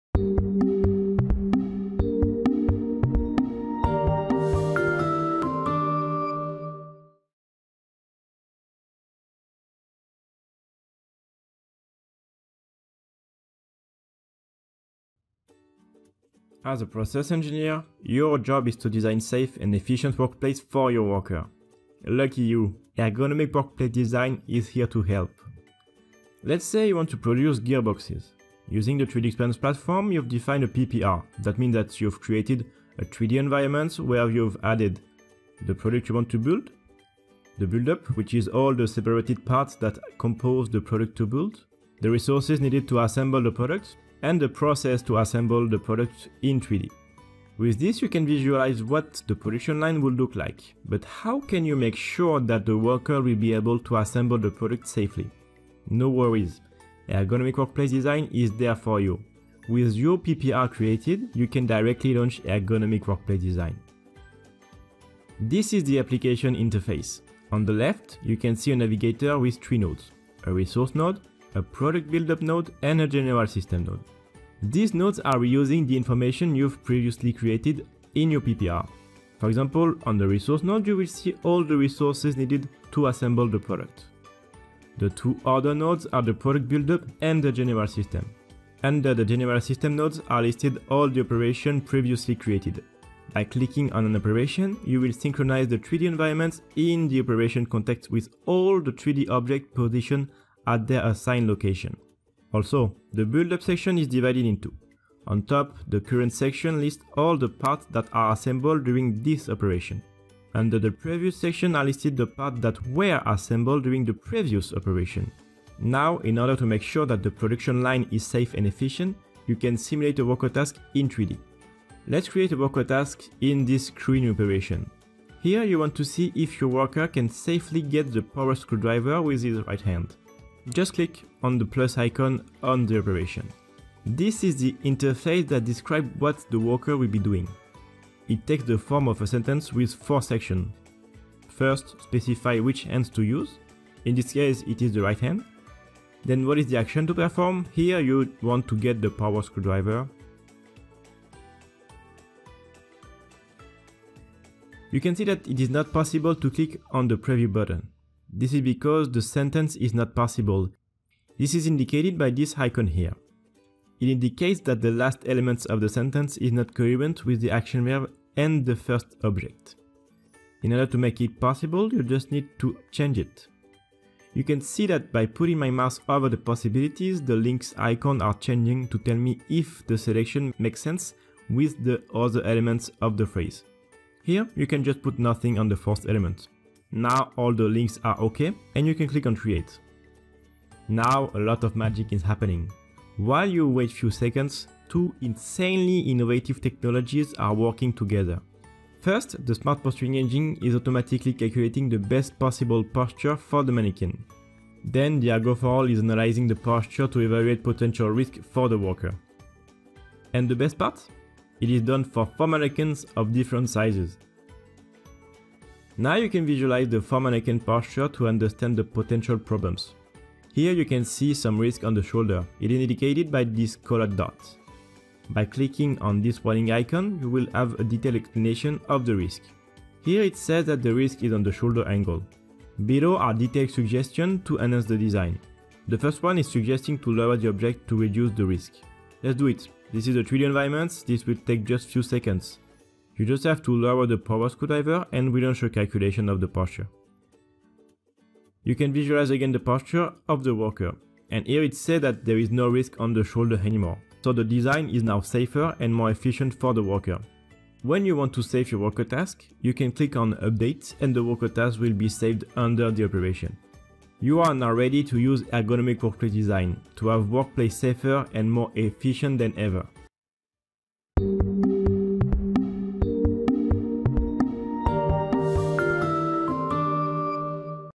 Design Ergonomic Workplace in 3D | DELMIA
This application represents a breakthrough for process engineers to design safe and efficient workplace for their workers in 3D.